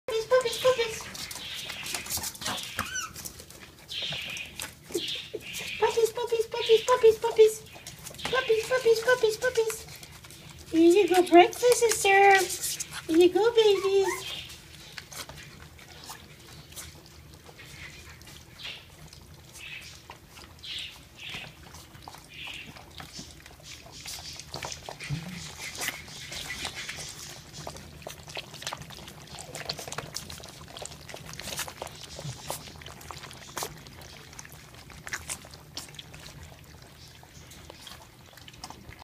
Chia's puppies waking up for breakfast
One month old American Hairless Terriers having breakfast.